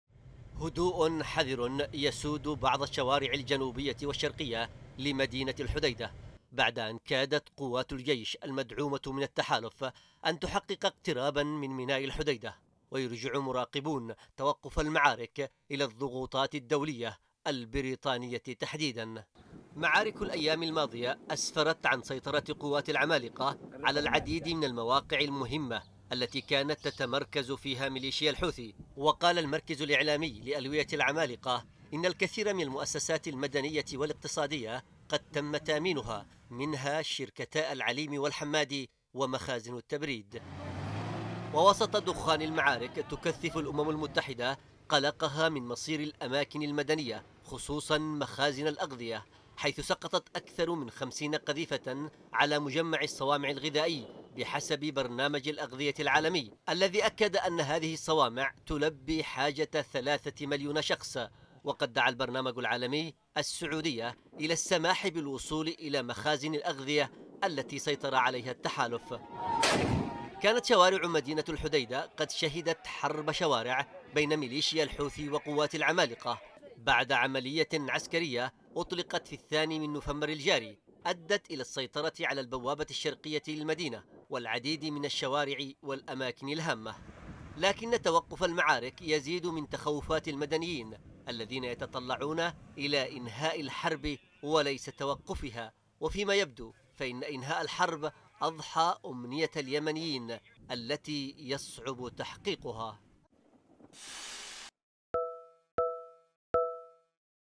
ضغوطات دولية توقف تقدماً عسكرياً لقوات الجيش باتجاه ميناء الحديدة | تقرير محمد اللطيفي
دعا برنامج الأغذية العالمي التابع للأمم المتحدة إلى السماح له بالوصول إلى مخازن رئيسية للأغذية سيطر عليها التحالف في الحديدة.
جاء ذلك خلال مؤتمر صحفي عقده المتحدث باسم البرنامج هيرفيه فيرهوسيل، في مكتب الأمم المتحدة بجنيف السويسرية.
وأوضح أن الصوامع تخزّن واحدا وخمسين ألف طن من القمح، وتلبي حاجة ثلاثة ملايين وسبعِمائة ألف شخص، مطالباً التحالف بالسماح بالوصول إليها في أقرب وقت، لكي يتمكن من مباشرة أنشطته بأمان